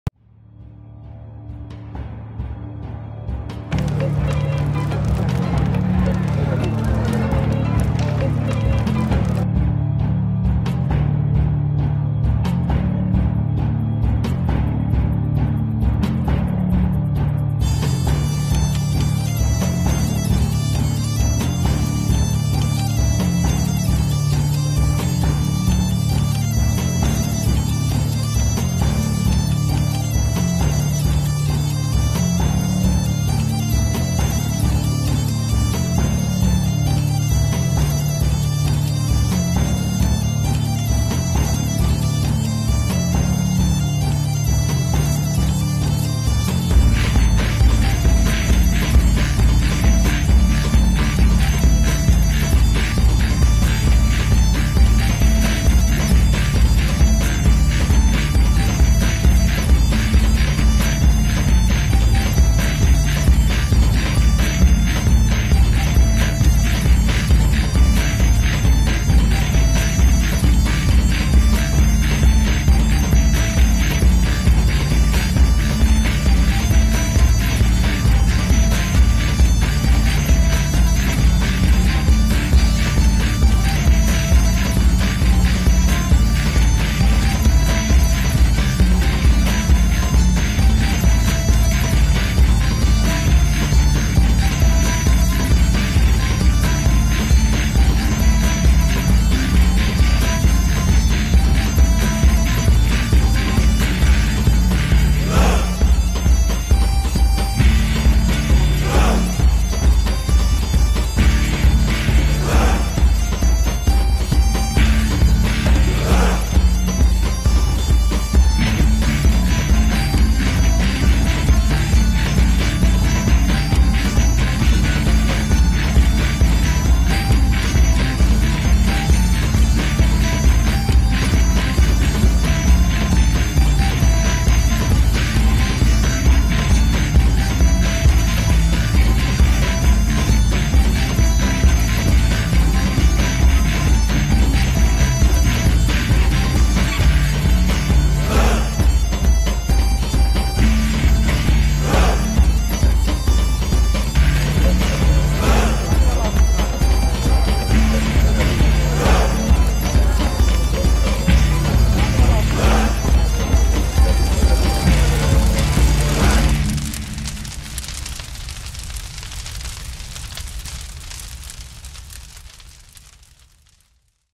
"La notte dei falò e dei desideri" | Trivigno 2014
"La notte dei falò e dei desideri 2014" | Festività di Sant'Antonio Abate - 18 Gennaio 2014

Continua il giro per i paesi lucani lucaniadascoprire2014, tappa a Trivigno(Pz) dove si festeggia nei pressi di un grandissimo falo' con piatti tipici, musica e balli. Presenti alla serata Pietro Cirillo e i tamburi dei Briganti. Bellissima serata, ci siamo divertiti molto ed abbiamo mangiato benissimo, si ringrazia la proloco di Trivigno per l'ottima organizzazione. L'anno prox tutti a Trivigno!

Videografo: Rocco Figliuolo
2a Camera: Enrico Di Bello
Shot: Canon 5d Mark III, Canon 5d Mark II, 14mm 3.1, 50mm 1.2
Edit: Fcpx